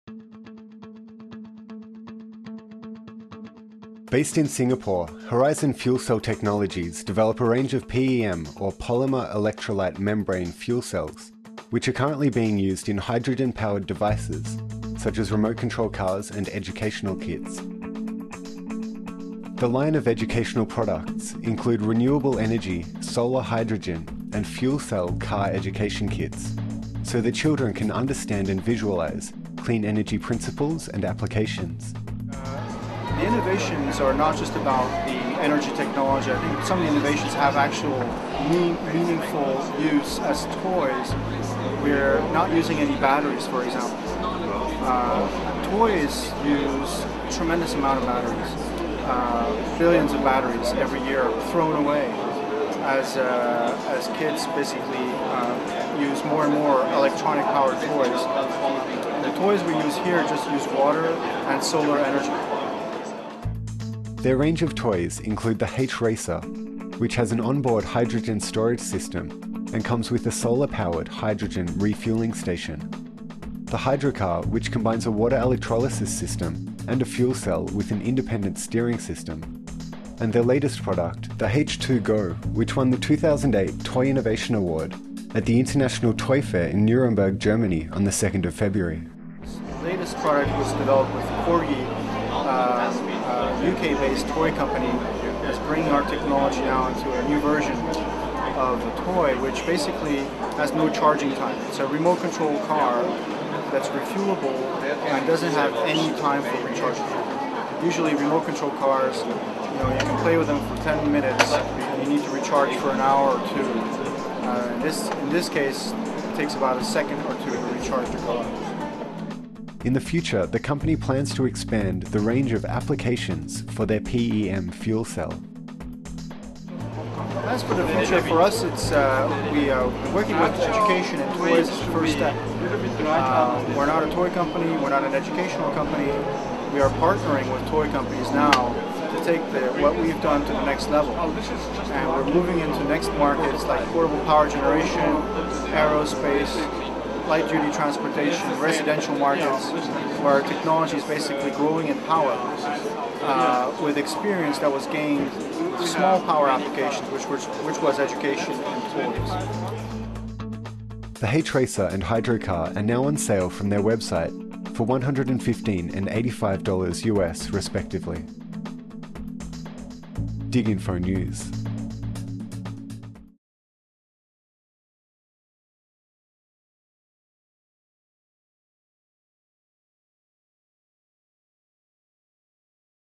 Horizon PEM Fuel Cells : DigInfo
Based in Singapore, Horizon Fuel Cell Technologies develop a range of PEM or Polymer Electrolyte Membrane fuel cells which are currently being used in hydrogen powered devices such as remote control cars and educational kits. The line of educational products include renewable energy, solar hydrogen and fuel cell car educational kits so that children can understand and visualize clean energy principles and applications. Their range of toys include the H-Racer, which has an on-board hydrogen storage system and comes with a solar powered hydrogen re-fueling station, the Hydrocar, which combines a water electrolysis system and a fuel cell with an independent steering system, and their latest product, the H2GO which won the the 2008 Toy Innovation Award at the International Toy Fair in Nuremberg, Germany on the 2nd of February. In the future the company plans to expand the range of applications for their PEM fuel cell. The H-Racer and Hydrocar are now on sale from their website for US$115 and US$85 respectively.